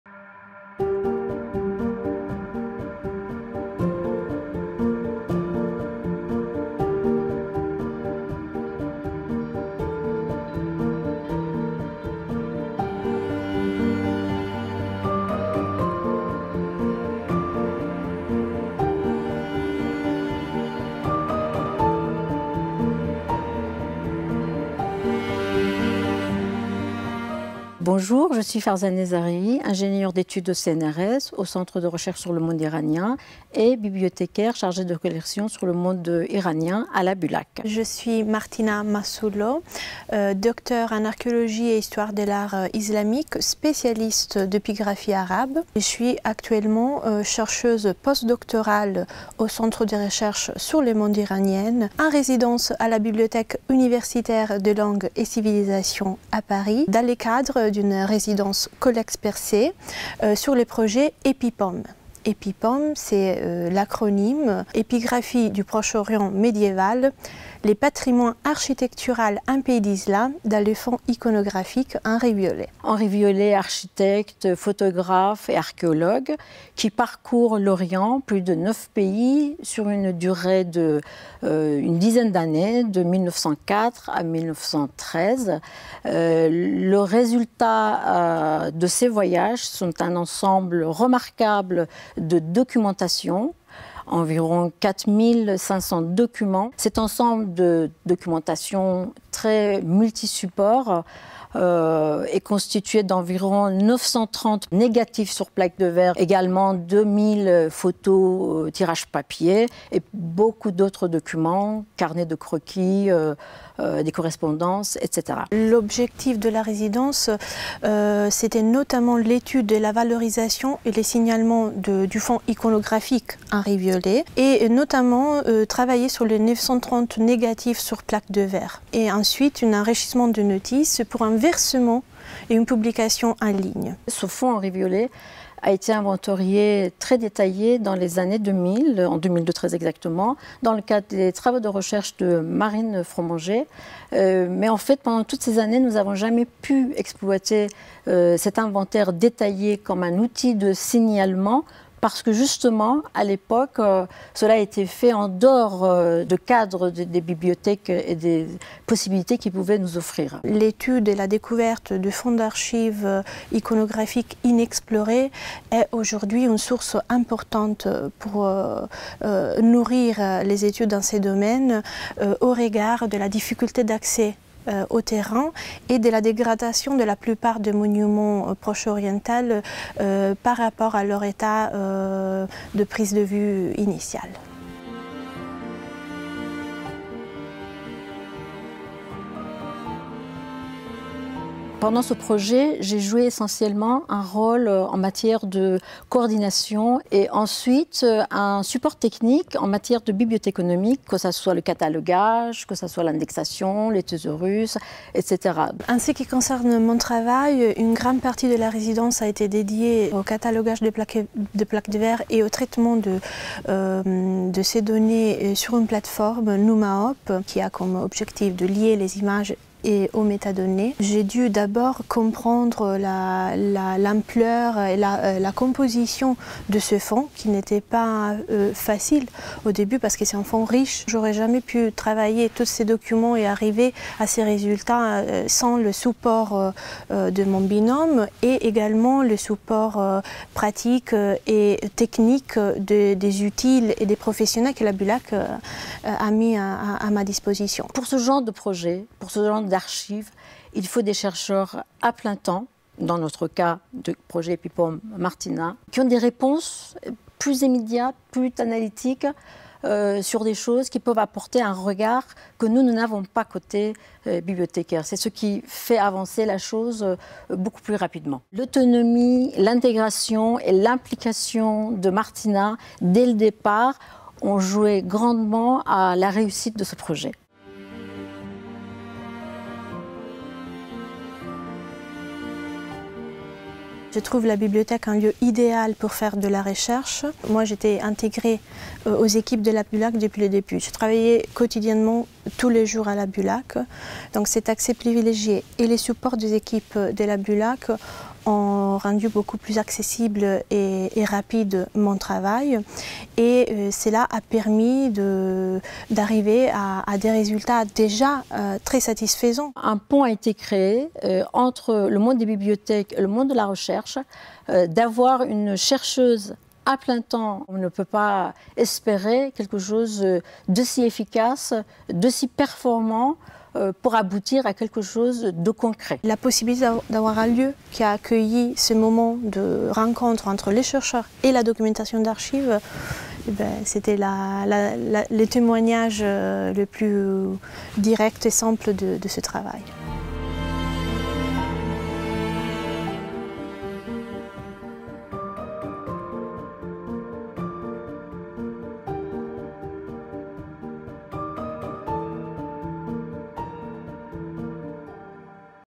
EpiPOM
Le patrimoine architectural en pays d’Islam dans le fonds iconographique Viollet

Résidences CollEx-Persée

Martina Massullo, Chercheuse en résidence - CeRMI-UMR 8041

Farzaneh Zareie, Ingénieur d’études, chargée de collections pour le domaine persan – BULAC

Le fonds envisagé est protéiforme et compte des photos (papier et plaques de verre), des archives, des documents. La BULAC propose un vrai environnement et accompagnement à la chercheuse pour son exploitation. L’intervention reviendra sur les enjeux scientifiques d’un traitement coordonné d’un tel ensemble, sur l’approche choisie pour documentariser le corpus ou encore sur l’instruction de la gestion des droits, essentielle pour la diffusion future. L’enjeu est de préparer un projet ANR à travers un réseau de partenaires spécialistes du patrimoine monumental islamique, consolidé à la faveur de la résidence. La journée d’étude prévue en conclusion, est cruciale pour éclairer l’importance du traitement de ce type d’archives scientifiques pour aider à la sauvegarde du patrimoine culturel islamique.